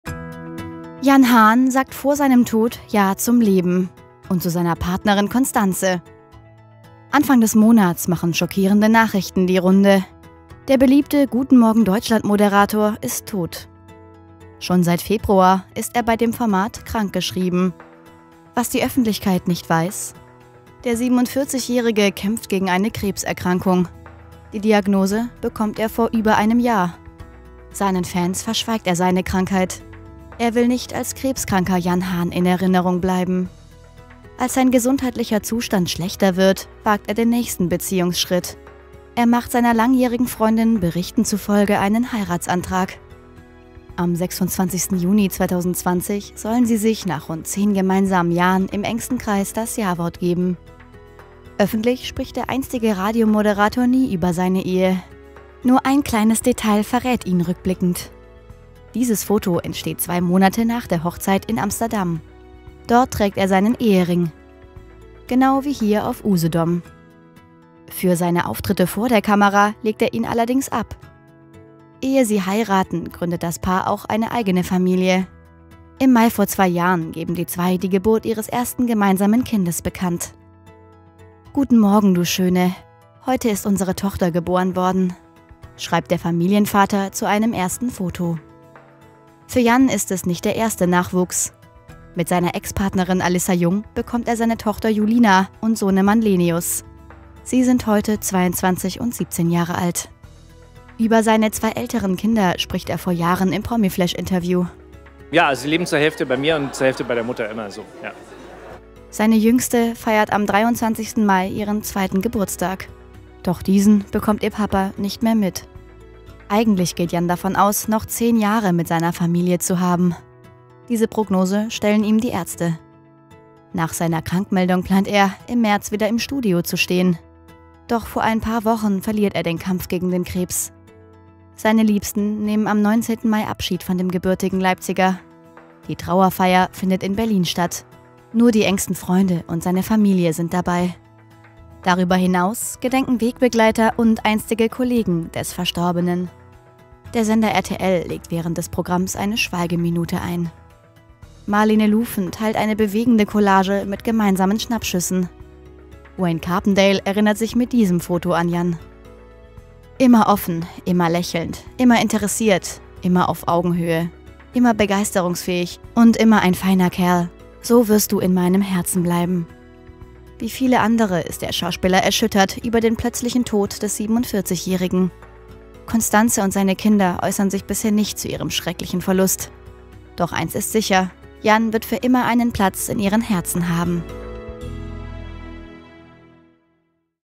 Er hielt es geheim: Jan Hahn heiratete kurz vor seinem Tod
Jan Hahn (✝47) hatte seine letzten Monate mit schönen Erinnerungen gefüllt. Anfang des Monats verstarb der "Guten Morgen Deutschland"-Moderator an den Folgen seiner Krebserkrankung. Als sich sein gesundheitlicher Zustand verschlechtert hatte, hatte er seiner langjährigen Freundin Constance Wendrich einen Heiratsantrag gemacht, wie Bunte nun berichtet. Bei einer intimen Zeremonie sollen sie sich im Juni vergangenen Jahres das Jawort gegeben haben. Seine Ehe hatte der gebürtige Leipziger vor der Öffentlichkeit geheim gehalten. Neben seiner Frau hinterlässt er ihre gemeinsame Tochter sowie zwei Kinder aus einer früheren Beziehung. (cko/kec)